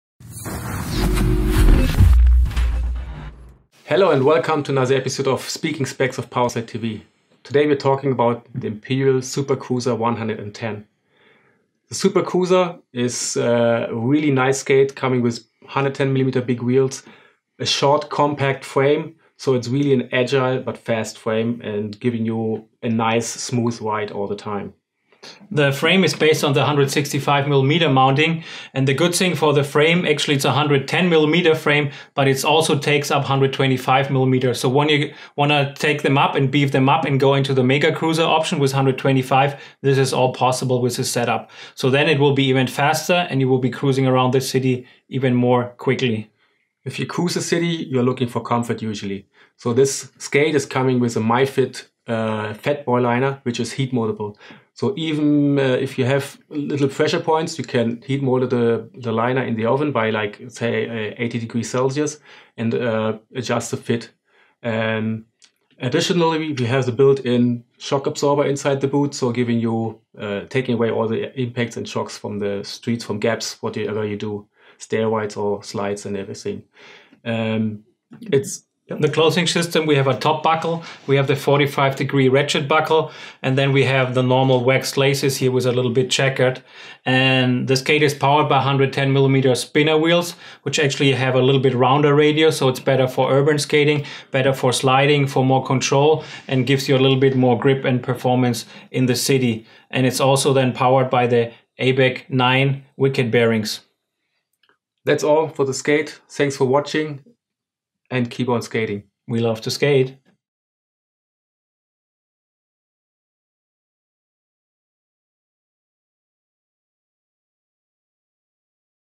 Powerslide Imperial Supercruiser 110 skates - Speaking Specs
Powerslide Imperial Supercruiser 110 skates - Powerslide Speaking Specs - 908227 - POWERSLIDE IMPERIAL SUPERCRUISER 110 – the classic city Supercruiser The Powerslide Imperial Supercruiser 110 is the obvious choice if you are looking for action and fun. The skate comes with 3x110mm wheels, our most popular Triskate setup. Fast and grippy 110mm/ 86A Powerslide Spinner wheels and Wicked ABEC 9 freespin bearings will make a skaters heart jumpstart. The rigid hard shell mounted on the Unity aluminum frame was designed with transportation & urban skating in mind. This commuter freeskate comes in a stylish white color way, with the comfortable and heat moldable MYFIT Fat Boy dual fit liner. Sizing problems are a thing of the past thanks to the stretch toe area of the liner which fits comfortable like a sock. The Powerslide Imperial 110 is more than an inline skate, it’s a true Supercruiser designed for the urban environment.